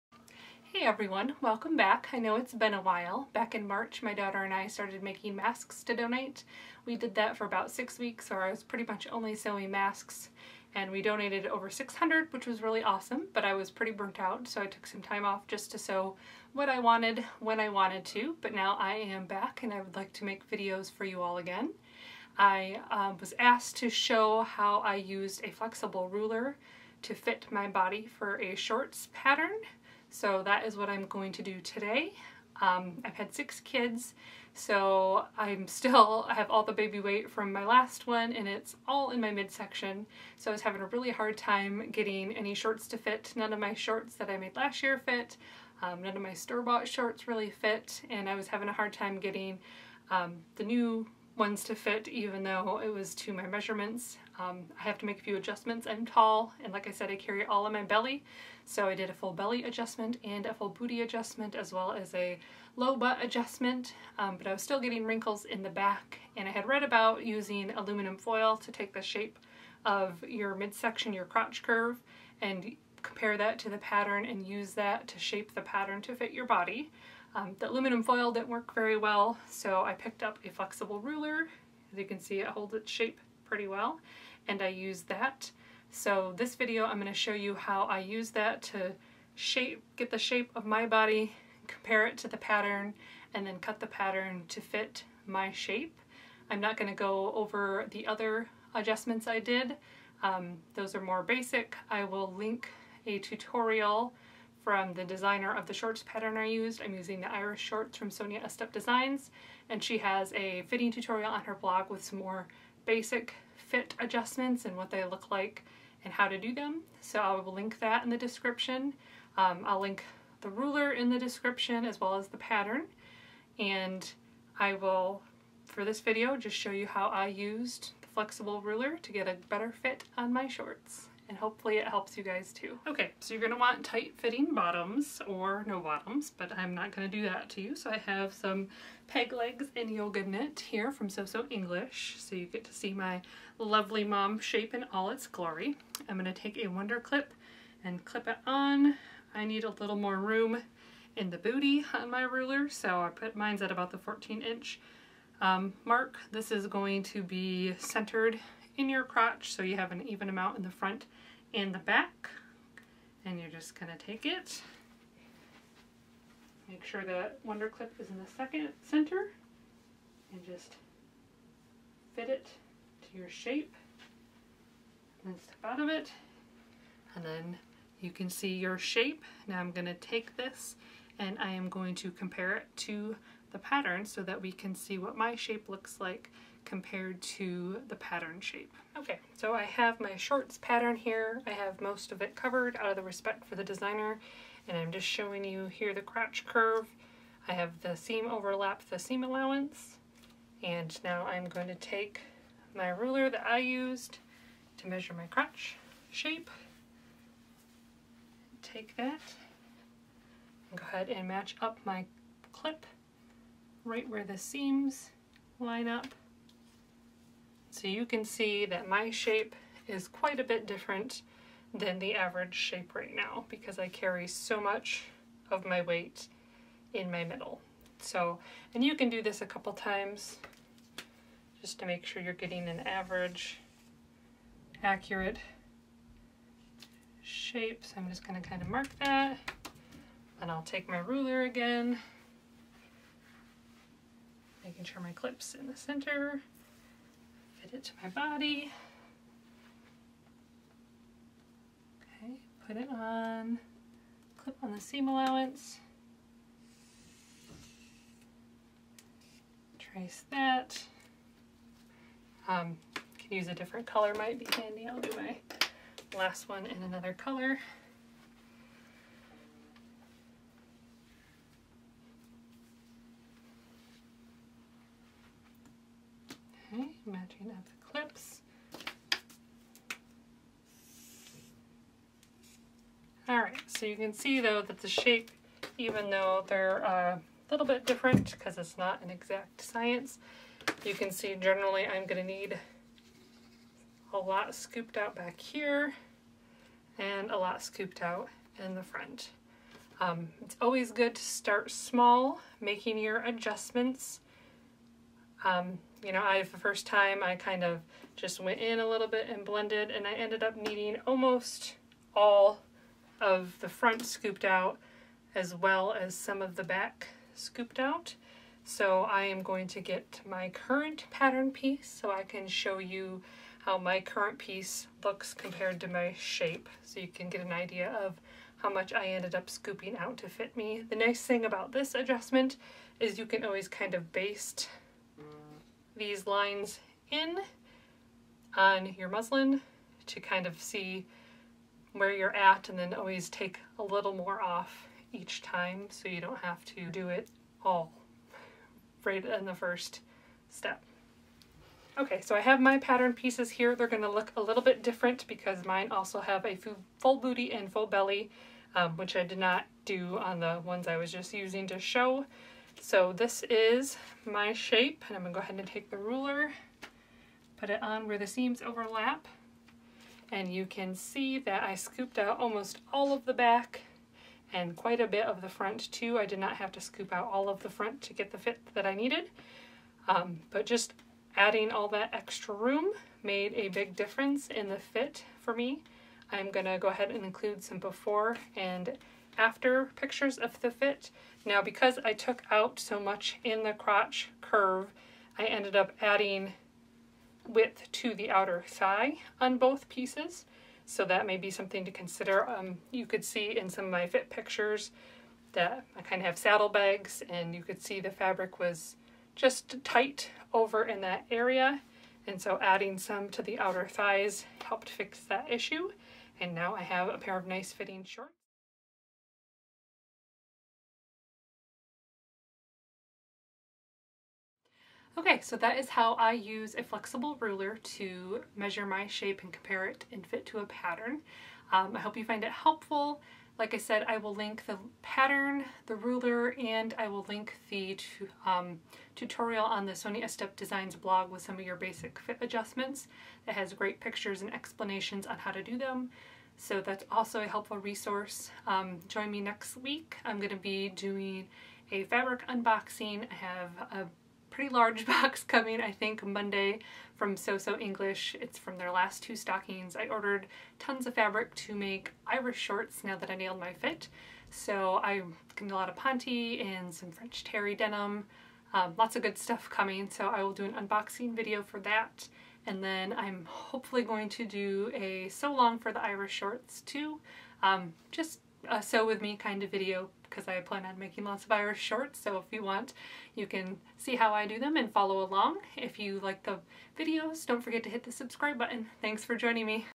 Sewing shorts - some fitting tips
I had been struggling getting a good fit with shorts on my postpartum body, so I used a flexible ruler to help match the patterns crotch curve shape to my body's shape.  I used the Iris shorts from Sonia Estep designs.  You can find them here @shorts (aff) Sonia has a great fitting tutorial on her blog here @/16/bottoms-that-fit/  You can find the flexible ruler that I used here on amazon @ (aff) I hope this video helps you achieve a great fit too!